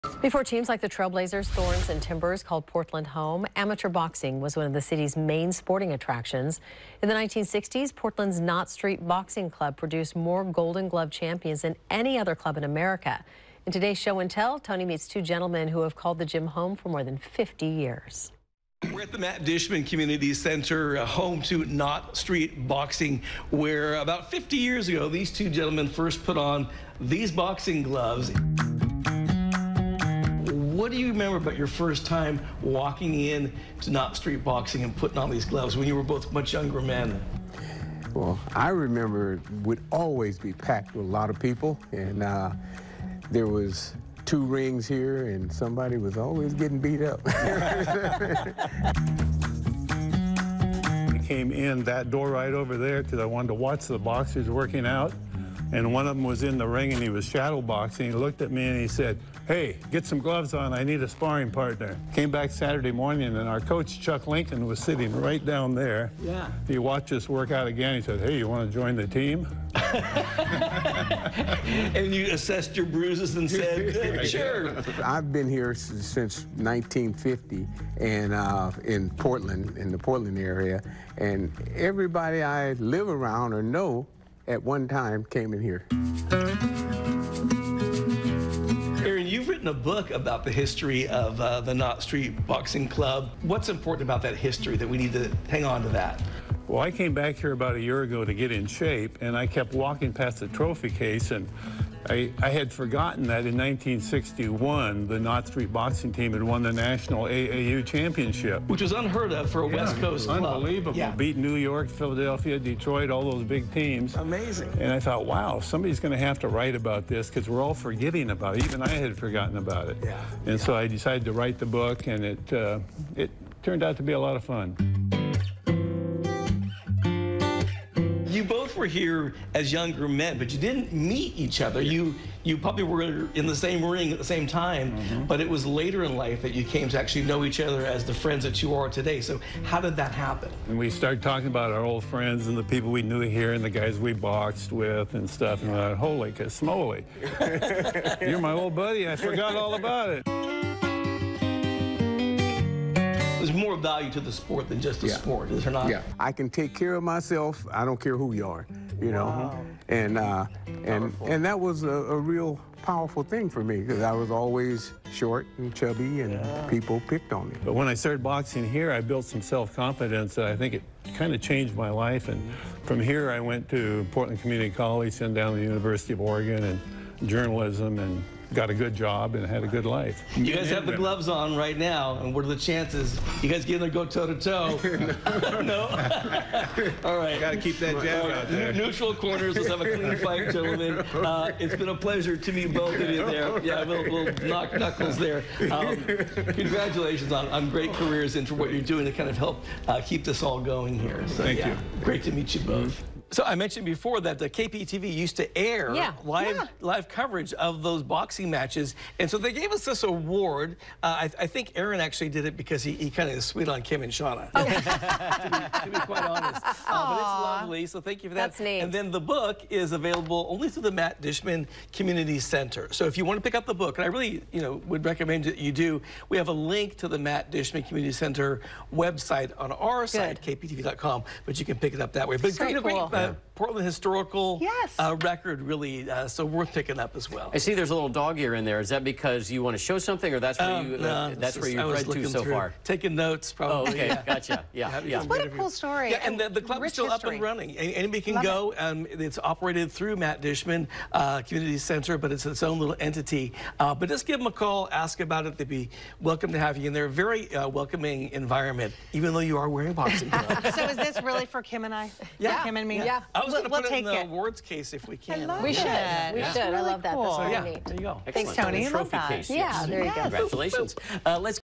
Show and Tell with Tony: Knott Street Boxing Club
In this Show and Tell, Tony Martinez meets two men who have called Knott Street Boxing Club home for more than 50 years.